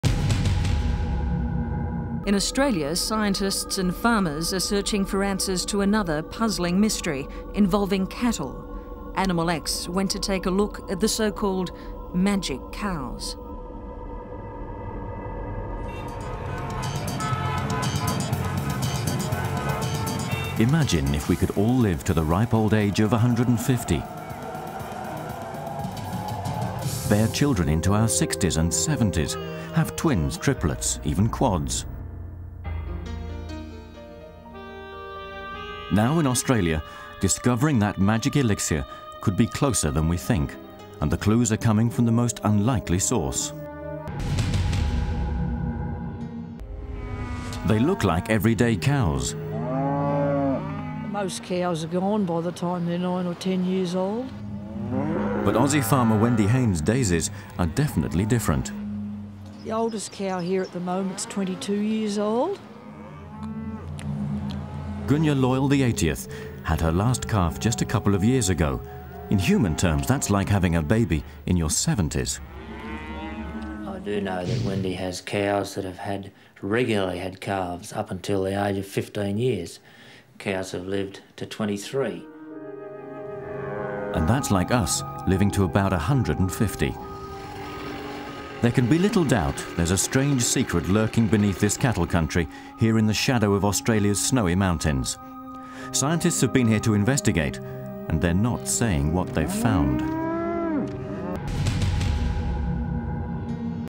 Immortal Cows?! Aussie Cattle Living 3x Longer & Still Having Calves! (Scientists BAFFLED!)
Have you ever heard of a cow living to 150?    Meet Wendy Hayne, an Aussie farmer baffled by her herd's supernatural longevity!  Some of her cows are defying all odds, living 3x longer than their kind and still giving birth in their golden years.

Scientists are baffled!  What's the secret behind these immortal cows living near Australia's Snowy Mountains?  Is it the magical water they drink?  A genetic anomaly?  Join our investigation as we delve into this mind-blowing mystery of super-powered cattle! ✨